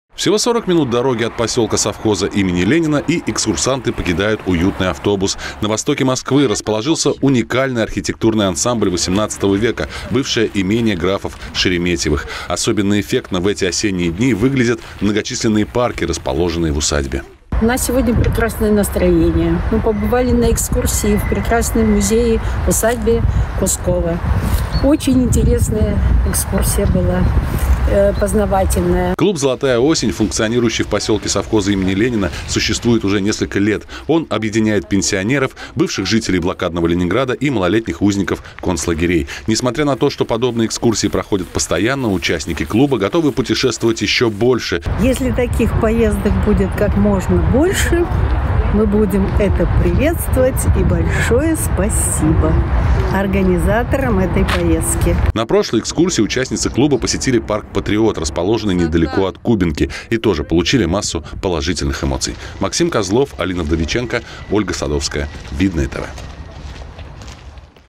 ЗОЛОТАЯ ОСЕНЬ
Для участников клуба «Золотая осень» посёлка совхоза была организована очередная экскурсия. На этот раз они отправились в усадьбу «Кусково».